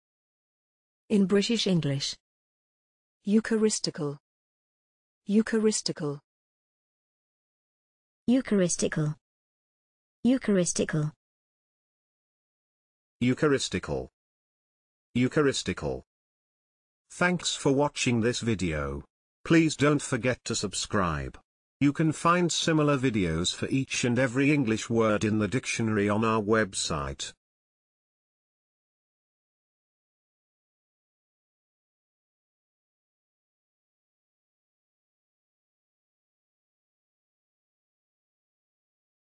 eucharistical - pronunciation
This is the pronunciation of eucharistical in British